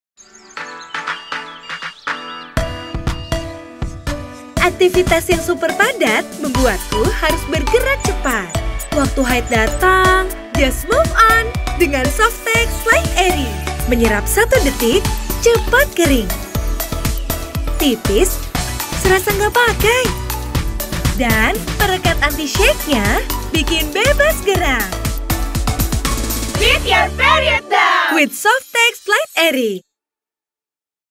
TVC Softex Light Airy – Just Move On 30s
Saat aktivitas padat, jangan sampai haid menghambat,

JUST MOVE ON dengan Softex Light Airy !